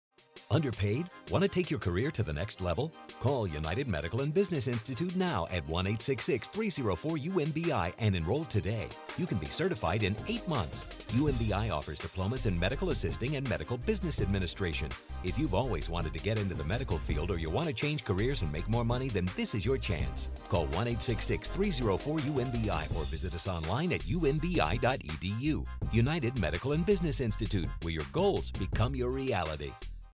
UMBI Commercial (TV) (Get Certified In 8 Months)
Start your new career in less than a year. Get certified in 8 months at United Medical and Business Institutes.